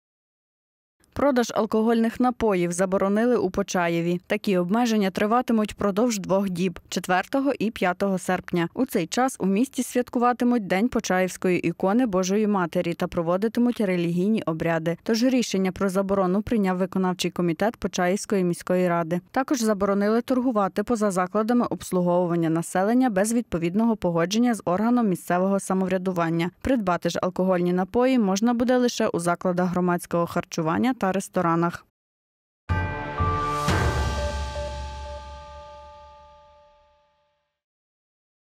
У Почаєві дві доби не продаватимуть алкоголь
Продаж алкогольних напоїв заборонили у Почаєві. Такі обмеження триватимуть впродовж двох діб - 4 і 5 серпня. У цей час у місті святкуватимуть День Почаївської ікони Божої Матері та проводитимуть релігійні обряди.